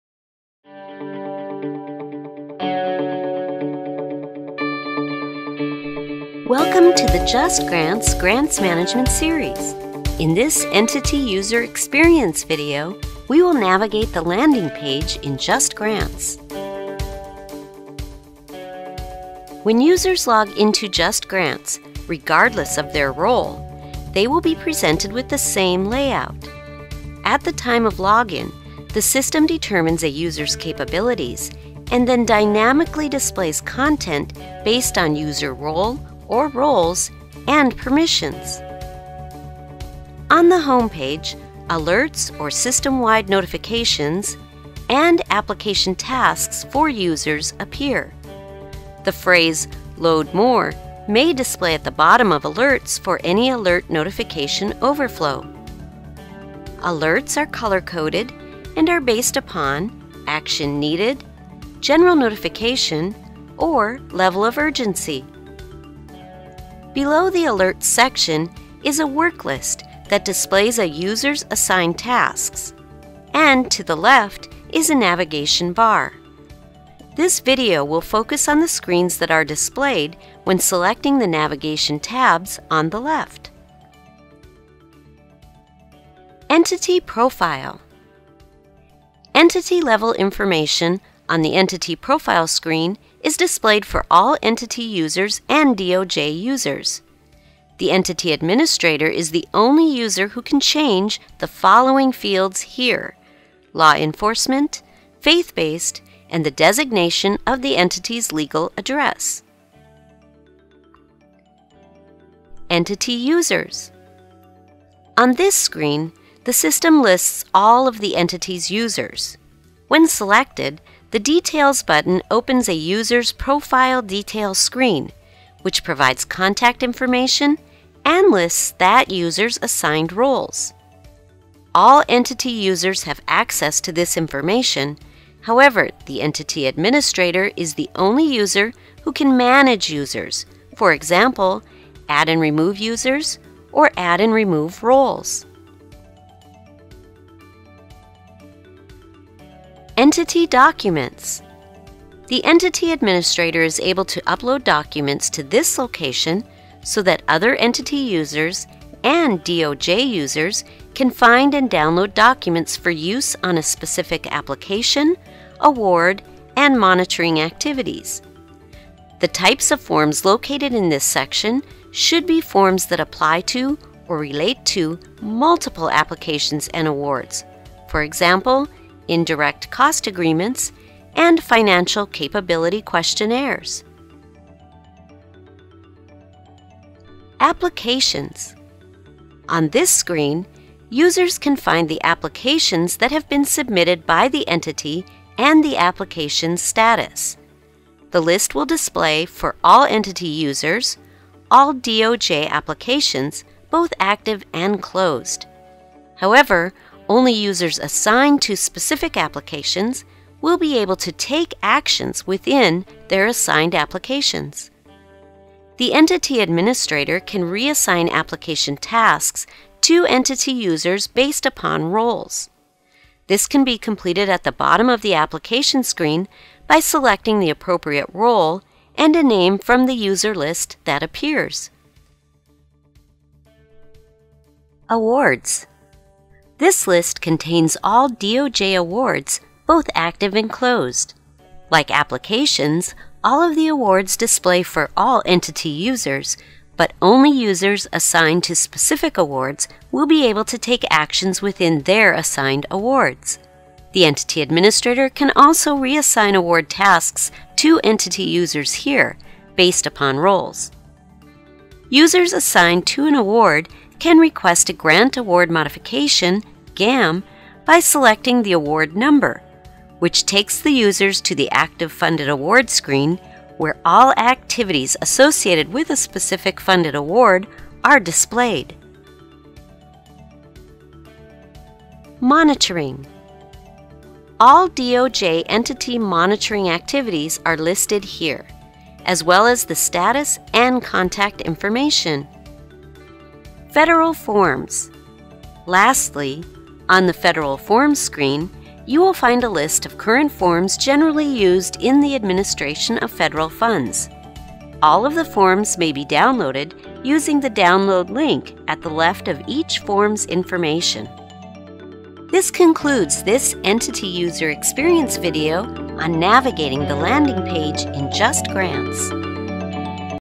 JustGrants Entity User Experience
Learn how to navigate the JustGrants Landing Page and access information available on the Home Page for taking action on work assigned to each user.